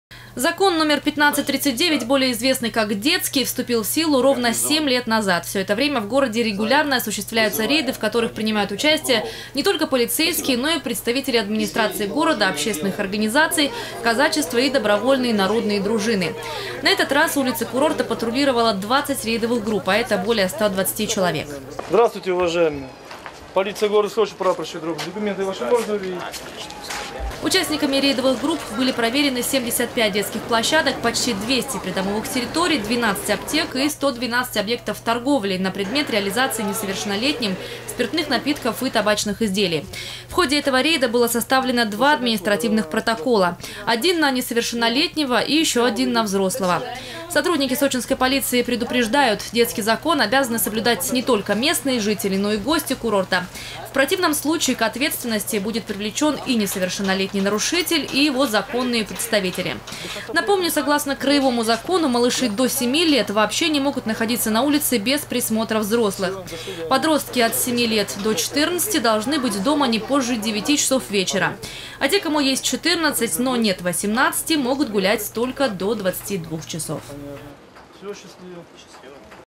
Как соблюдается «детский закон», проверили в Сочи. Новости Сочи Эфкате
По улицам центра курорта прошло 20 рейдовых групп.
Сочи. Новости.
Телеканал Эфкате.